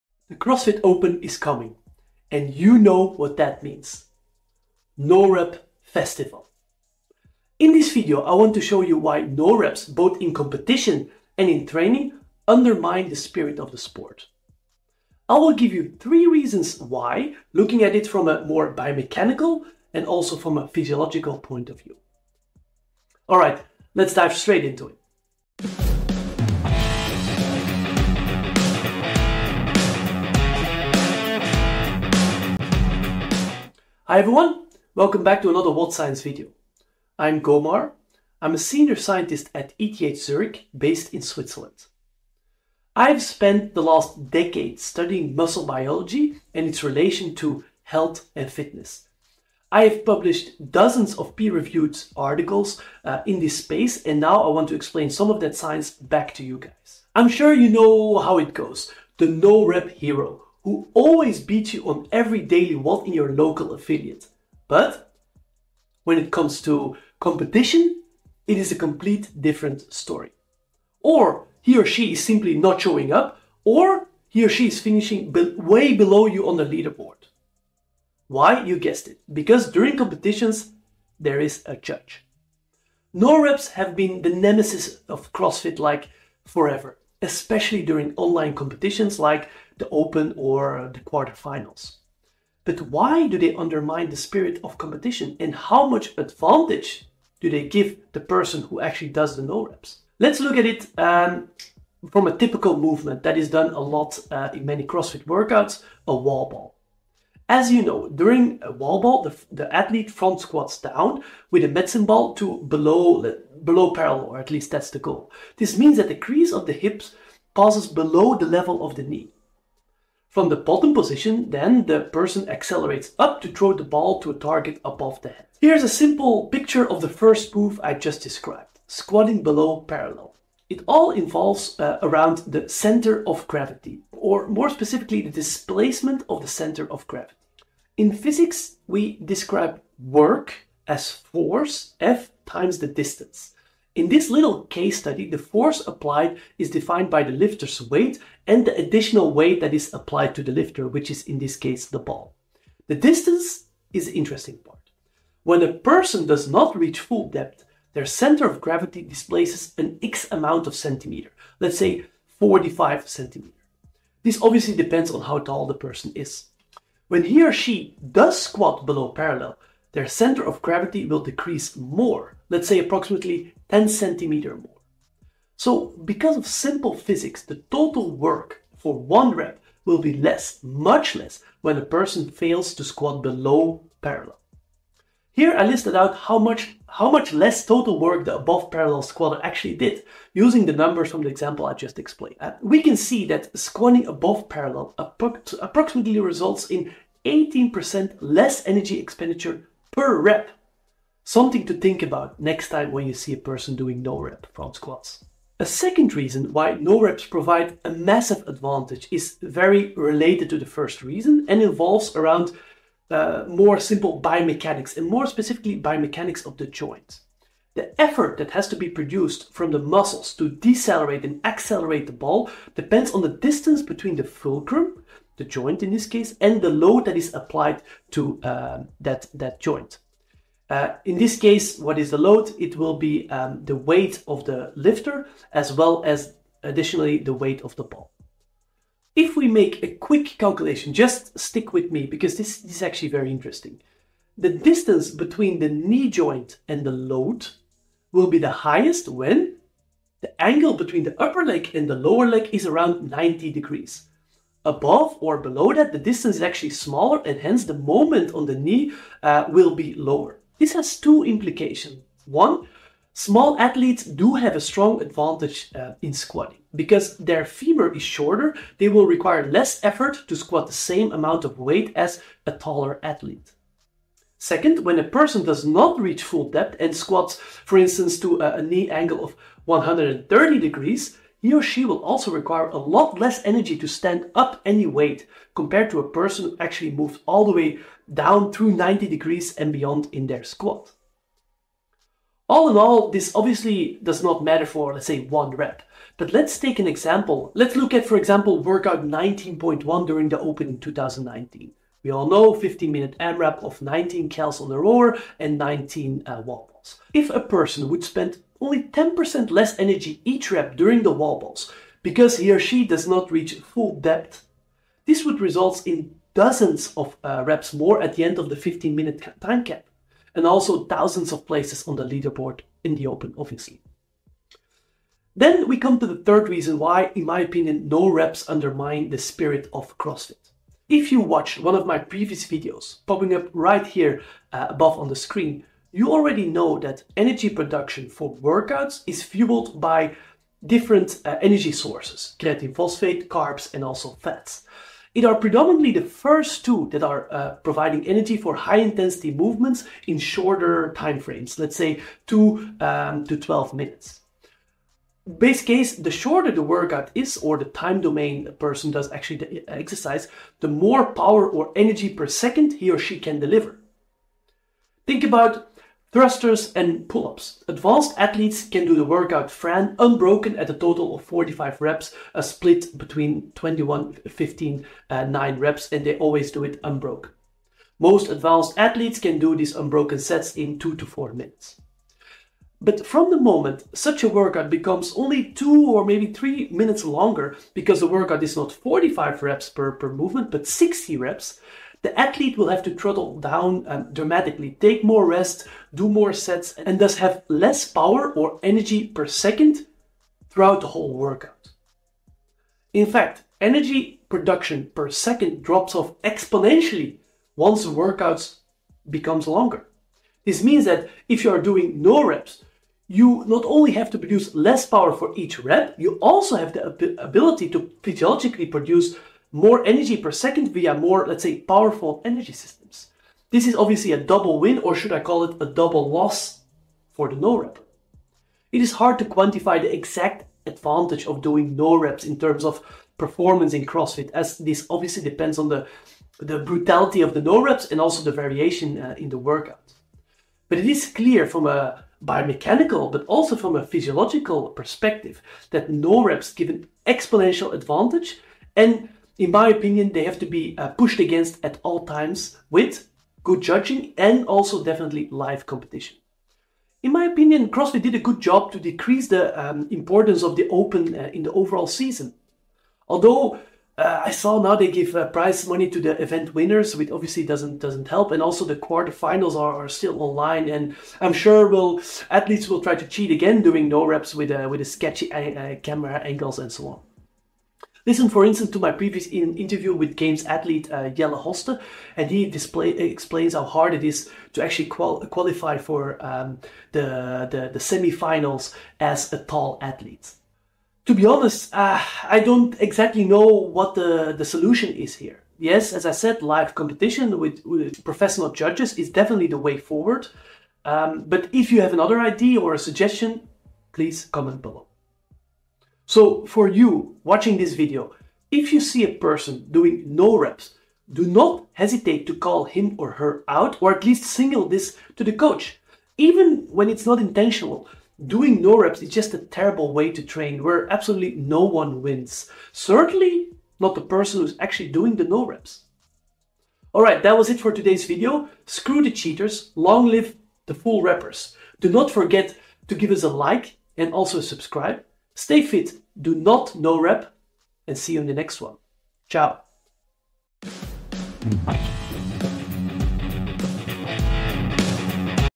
No Reps in CrossFit: The Hidden Impact on Athletes and the Sport
Next time you see someone doing no reps - show ‘m this video.

Since forever no reps have been the nemesis of CrossFit. In this video, I dive into the reasons why "no reps" in CrossFit actually provide the individual with a substantial advantage from both a biomechanical and physiological standpoint.

Timestamps and references below:
0:00 - Intro
1:37 - Why do No Reps give such an advantage in CrossFit
2:10 - Reason 1 - Displacement of the center of gravity
3:34 - Reason 2 - Biomechanics of the joint
5:30 - Reason 3 - Physiology and energy systems
8:10 - The solution?

FOLLOW US for FREE evidence based education on Fitness and Nutrition.